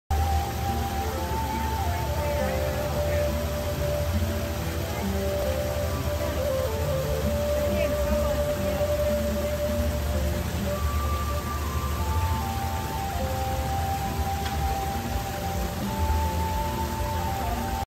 Waterfall in Japan Pavilion
listen to the relaxing music and sounds of the  waterfall in the Japan pavilion at Epcot